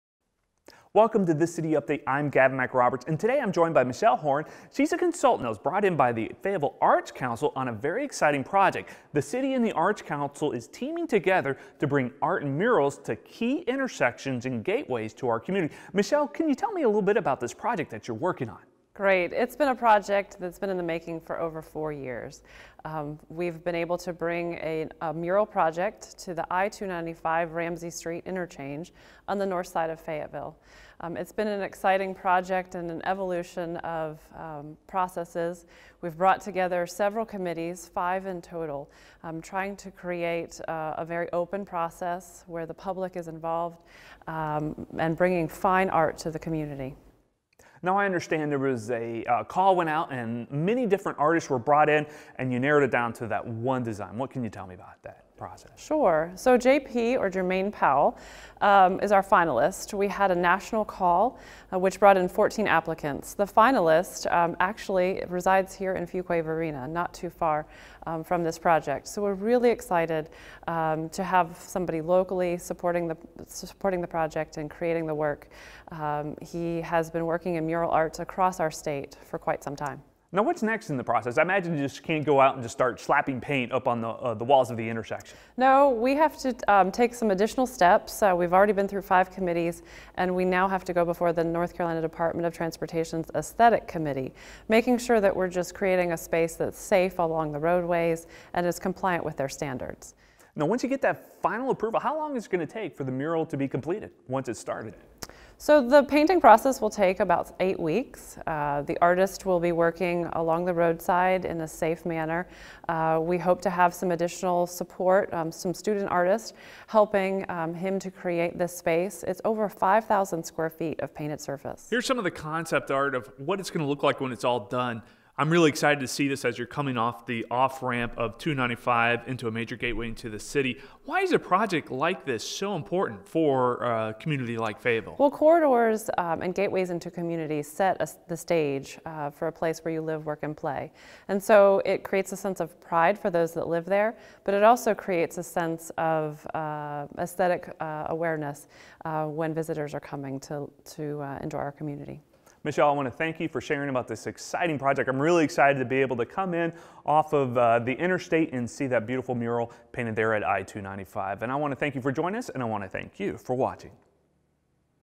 City Update- I-295 Mural
In this City Update, learn more about the new mural that will be painted at one of the gateways to Fayetteville.